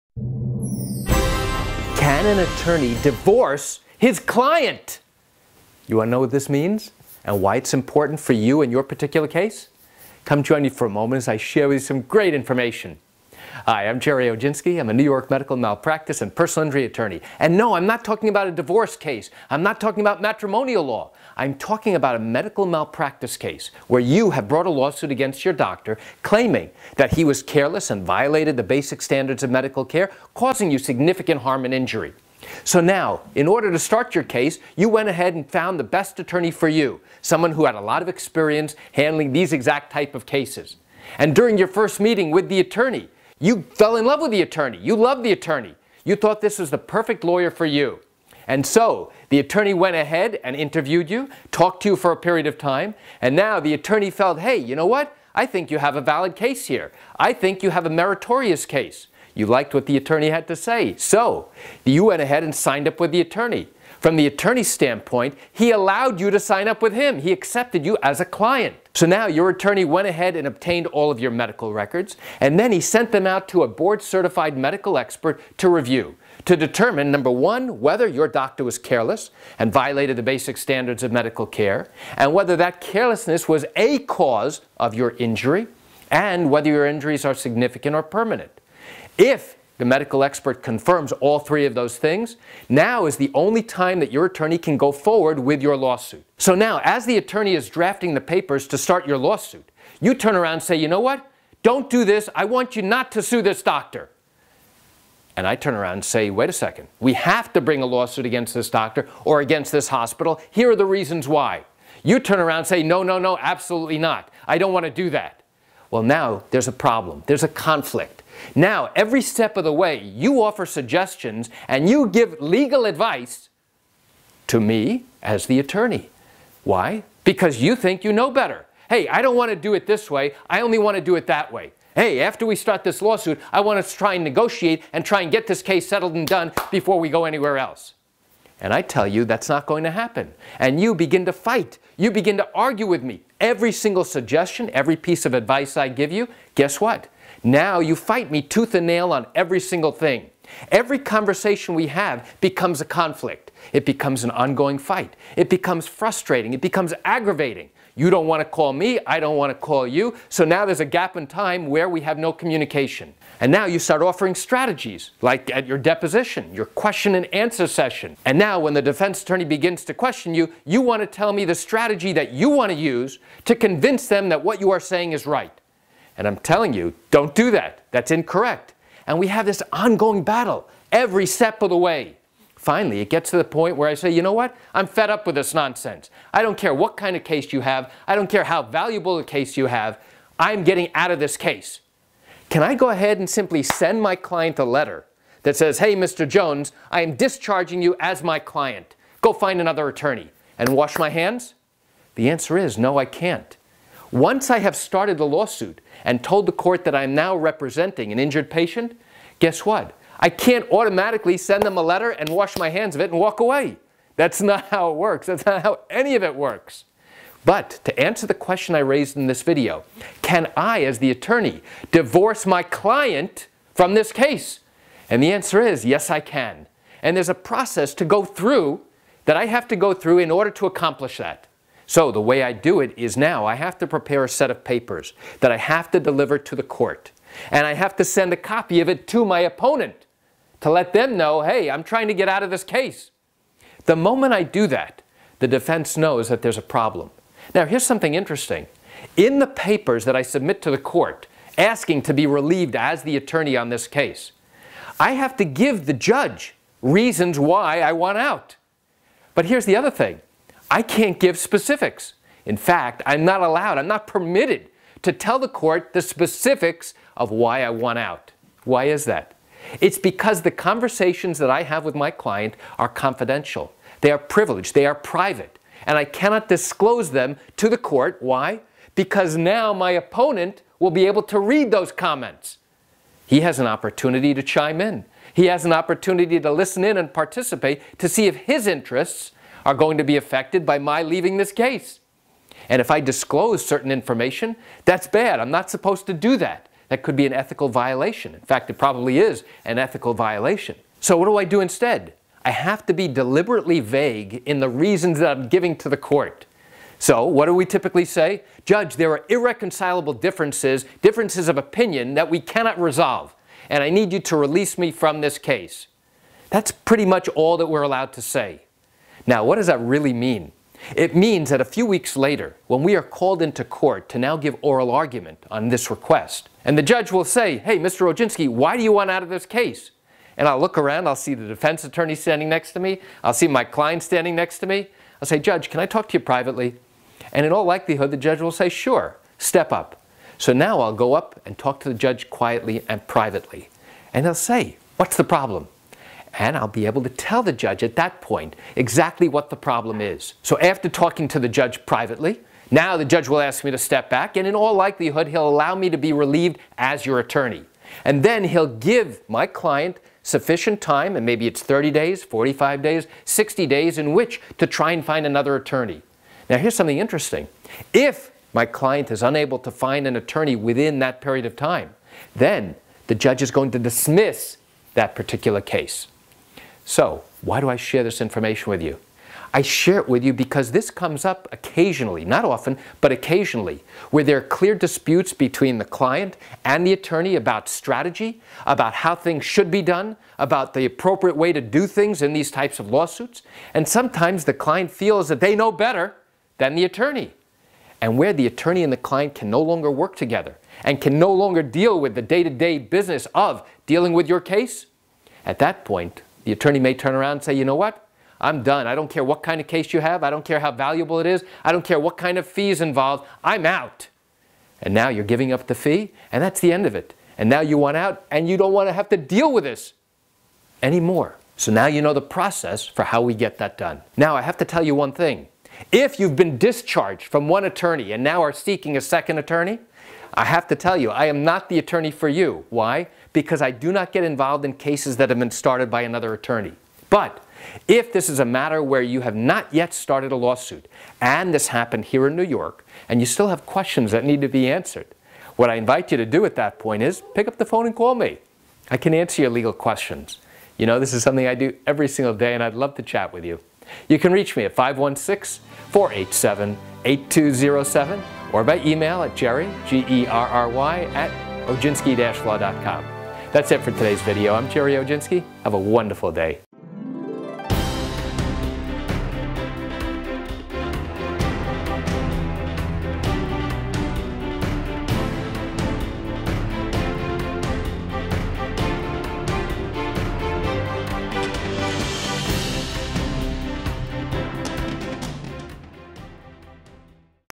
Can An Attorney DIVORCE His CLIENT When They No Longer Get Along in a Medical Malpractice Lawsuit?
NY Medical Malpractice & Personal Injury Lawyer Gerry Oginski Explains

This has nothing to do with divorce law.
It has nothing to do with matrimonial law.
It has everything to do with the marriage that happens when you sign up with a lawyer to handle your medical malpractice case and when your lawyer accepts you as a client.

I say 'marriage' because the two of you will be together for years.
In New York, a medical malpractice case takes anywhere from two to three years to reach a conclusion.

That means that you and your lawyer need to get along during that time. You need to be civil to each other. You need to respect each other.

If you're constantly fighting, that's no good.
If you think you know how to handle your case better than your attorney and continue to tell him so, that's not good.

It you continue to butt head with your lawyer about legal strategy, that's also not good.

Why not?
Because your lawyer may reach a breaking point.
You may be frustrated with his suggestions.

Your attorney may be aggravated with you every time he gets off the phone with you.
There may come a time when he throws his hands up in the air and says "I'm done. I want you and your case out of here! I no longer want to represent you. It's not worth it," he says out of frustration.

Can your lawyer actually 'divorce' himself from your case?
Can he tell you to go find another lawyer and be done with you?

Here's a cardiac malpractice case where I was able to achieve a $6 million dollar settlement for my client

To learn more about how accident & medical malpractice cases work in the state of New York, I encourage you to explore my educational